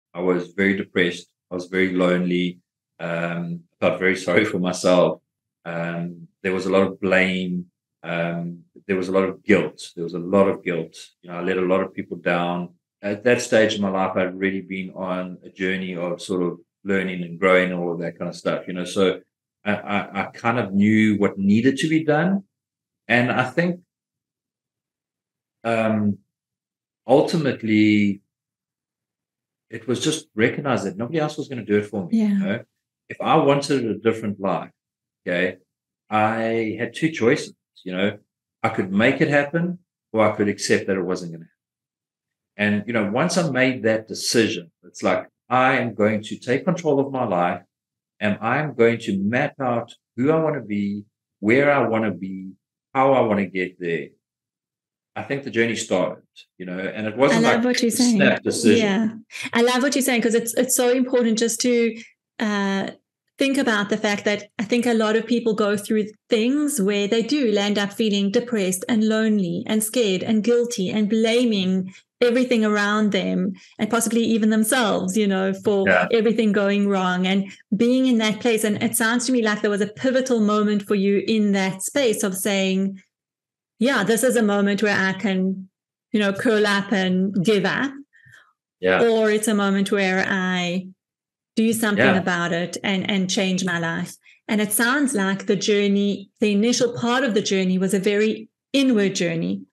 When guilt, anger and depression hits
When guilt, anger and depression hits you may want to blame people around you and carry the guilt. However, you can make the choice of whether you want to give up or create a different life and map out your path towards who you want to become.

So, will you settle for circumstances or take charge of your own destiny?